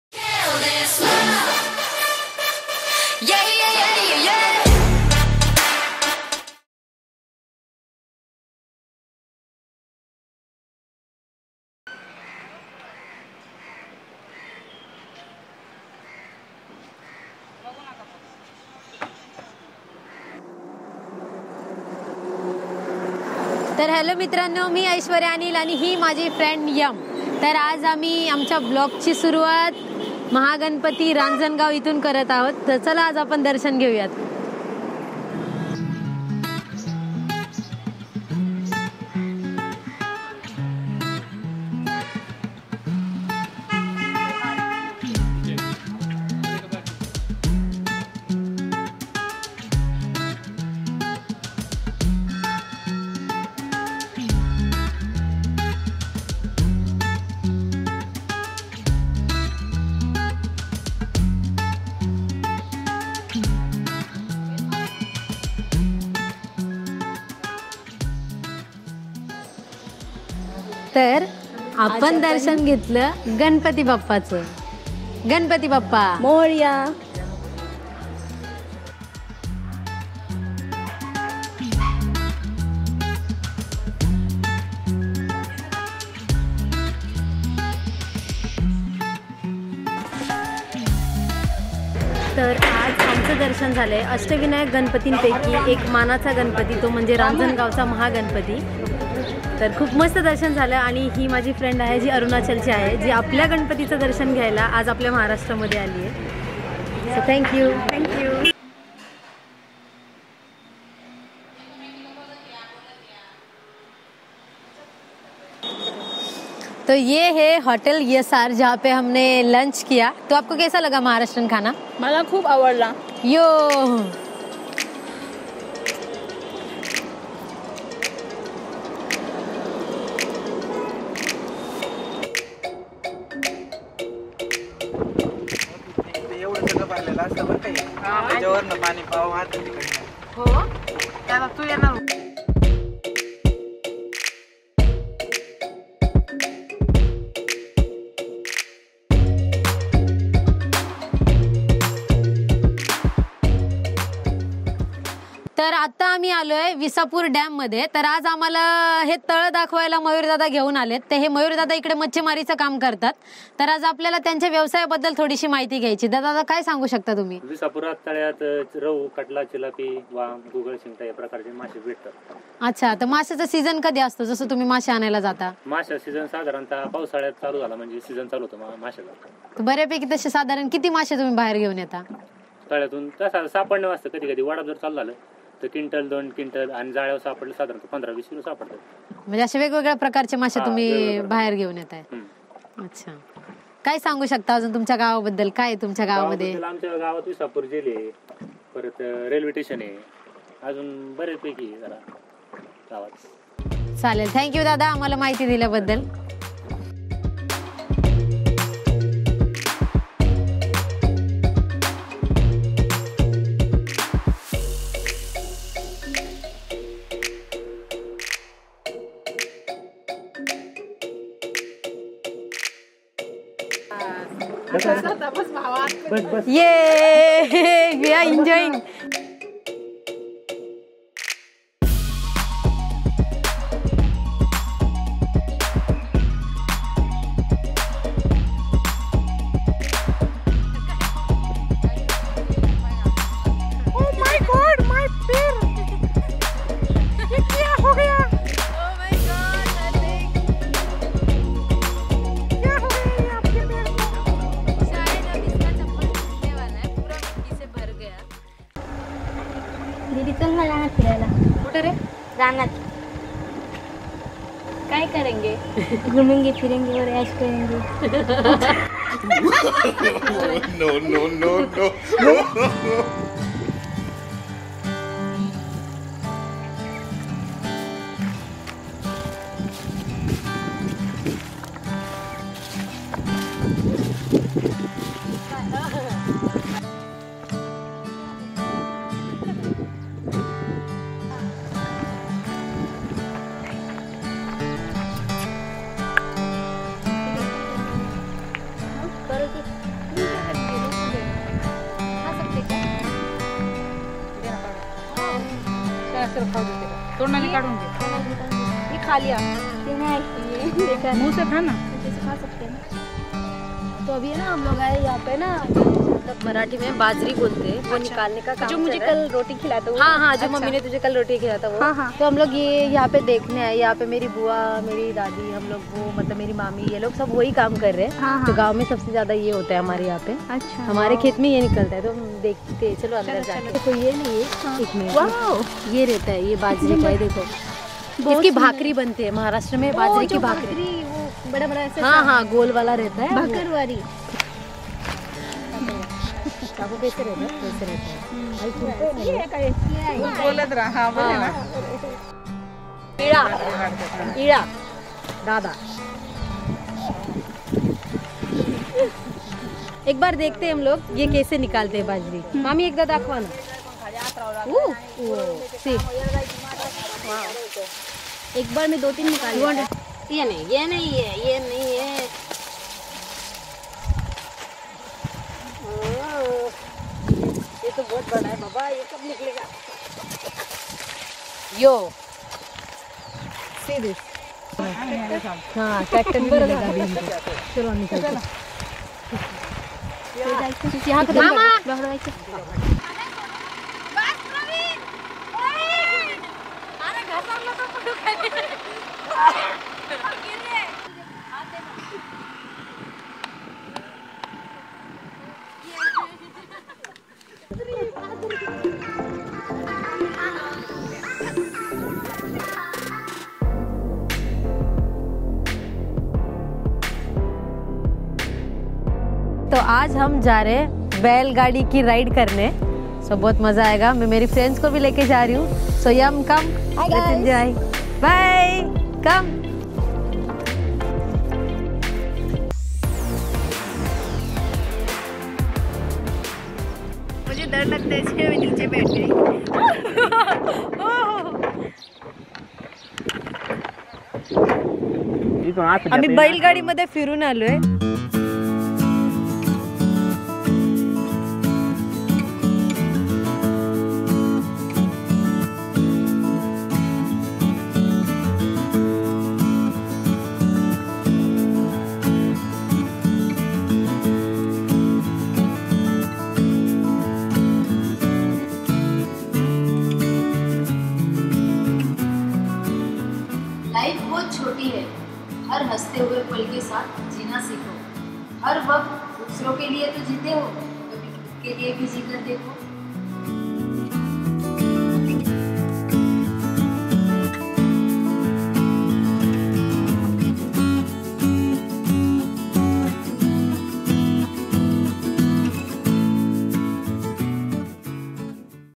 My First Vlog | Aishwarya Anil | Tabayamana | AA Creation
I am Happy to share my first Vlog.
we went to Ranjangaon, Visapur, Math Rajapur, Shirur, NImgaon to shoot our first Vlog. all people are so kind with us. and i am proud to say that here i am  represent our Maharashtra in this video.
its Collab Video with Tabayamana. She is my best friend and from Arunachal Pradesh. we had lots of fun and Proudly present our maharashtriyan culture.